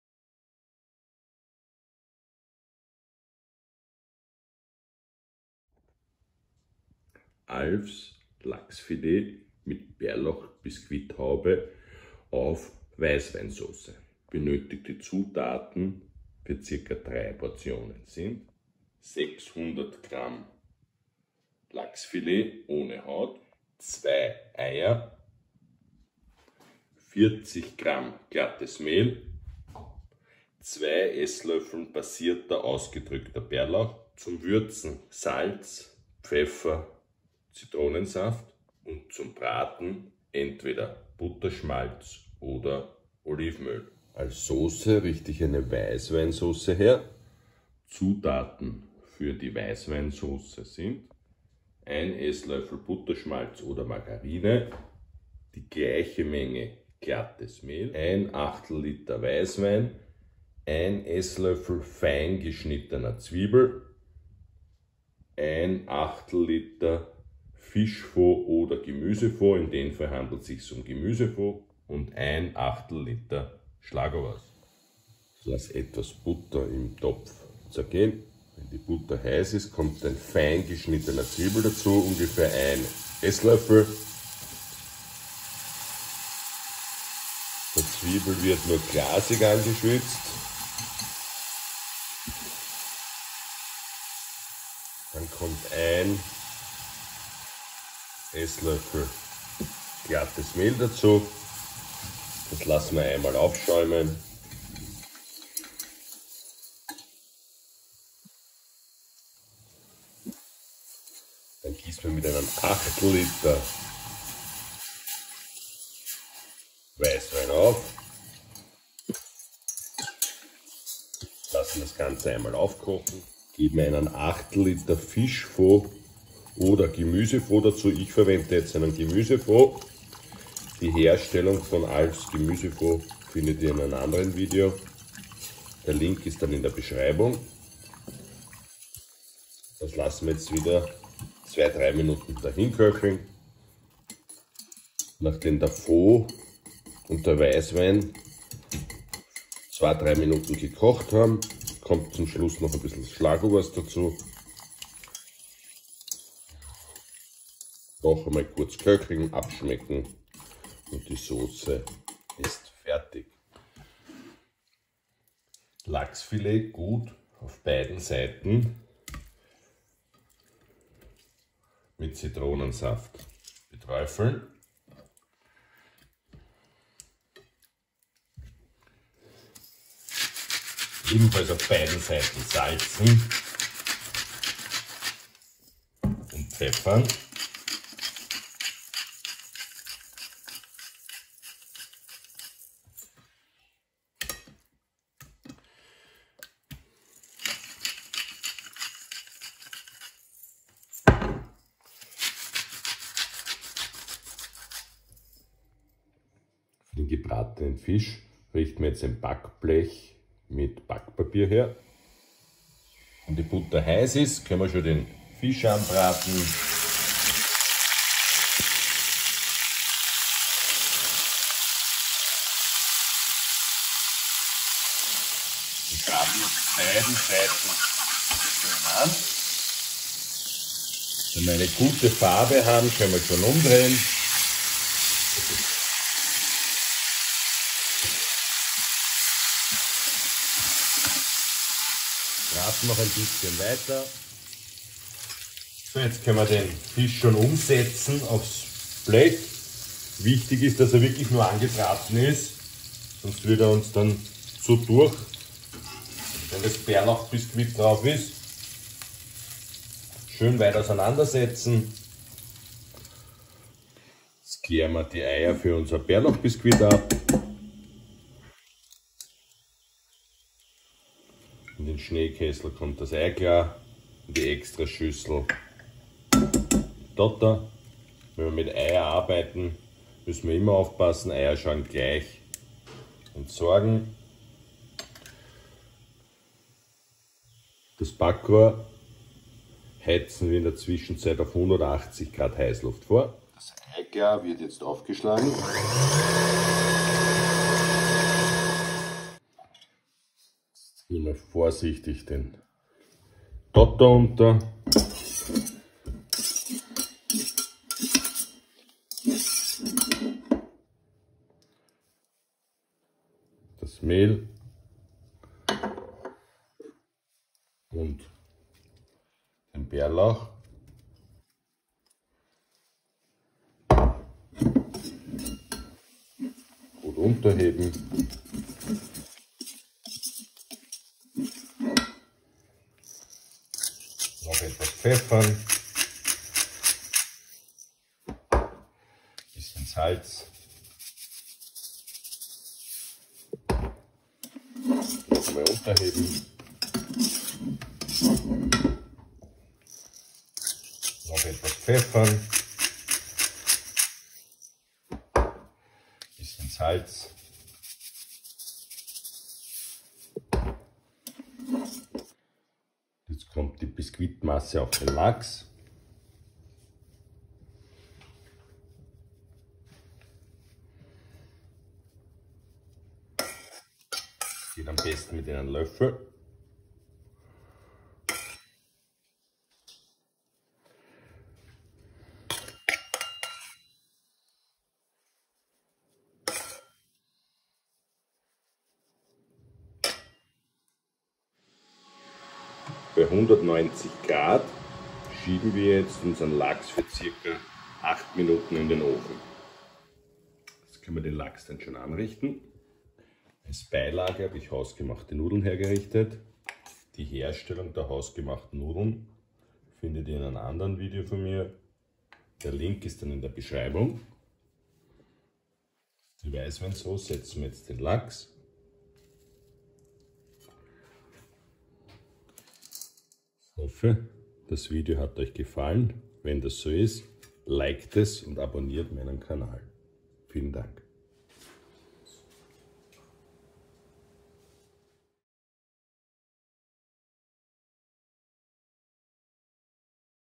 Lachsfilet mit Bärlauchbiskuithaube aus Alf`s Kombüse
habe ich wieder ein neues Video gemacht, dieses Mal geht es um Fisch mit Bärlauch.
Ich hoffe es gefällt. Lasst doch ein Abo, ein Like oder einen netten Kommentar da, wir würde mich freuen.

"Lachsfilet mit Bärlauchbiskuithaube auf Weißweinsauce"

Solltet Ihr besondere Wünsche zu neuen Videos haben lasst es mich wissen.